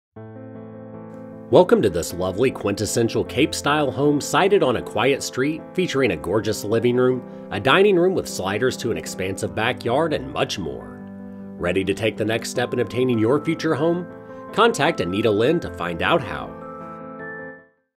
104 Bertwell Road Lexington, MA | ColdwellBankerHomes.com
104 Bertwell Road, Lexington, MA

Welcome to this lovely quintessential Cape style home sited on a quiet street with a generous-sized level lot. The moment you walk into this bright and sunny home, you will love the openness and the easy flowing interiors! It features a gracious fireplace, a spacious living room, which opens to the